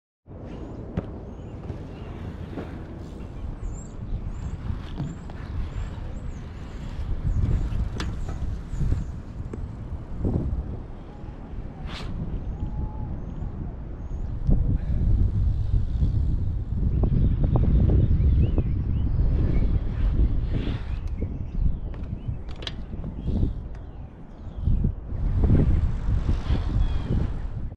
Lazy Sunday Stylin' on the 2020 Haro Sport Bashguard!
Out for some lazy Sunday freestylin' on the Sport.